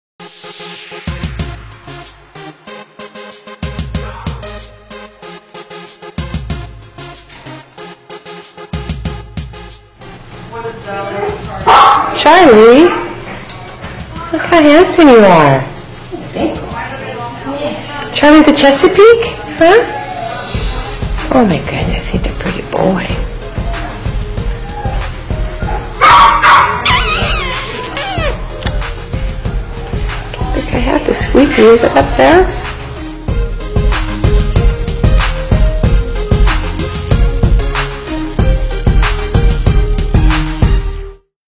Charlie
★ CHARLIE AVAILABLE Dog (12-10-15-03) Young • Male • Large★

CHARLIE is a large young male who is very intelligent and well behaved.

CHARLIE is up to date on shots and is available from Tehama County Animal Care Center.